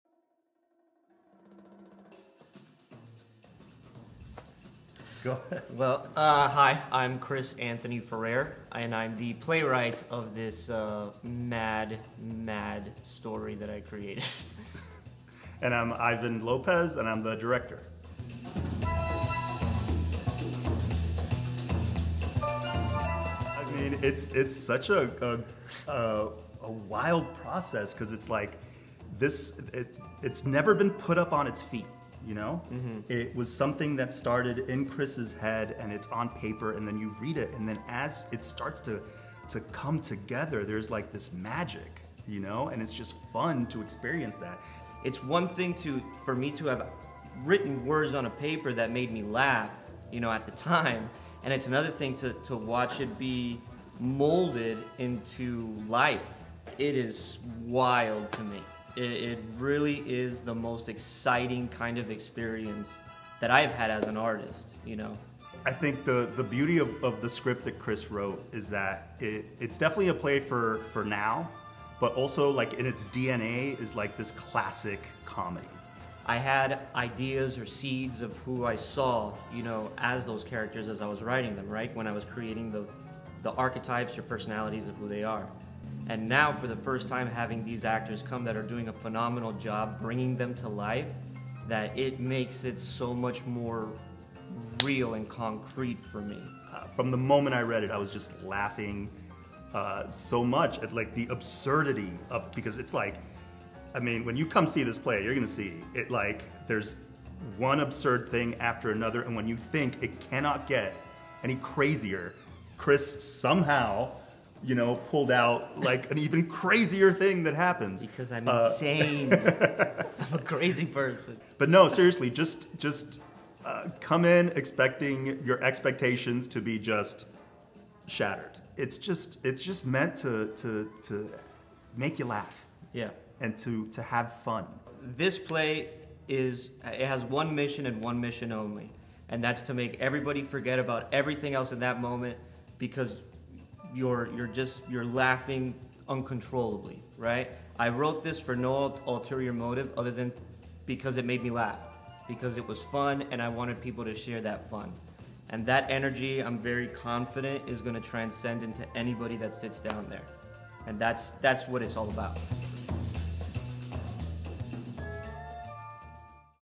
Swindlers Behind the Curtain (Ep. 1)
Behind the Curtain is a series designed to take audiences into the room where it happens. We will interview key players in the process of bringing a play or musical from the page to stage.

In this episode, playwright, Chris Anthony Ferrer & director, Ivan Lopez tell us their process in bringing SWINDLERS from page to stage for the first time ever.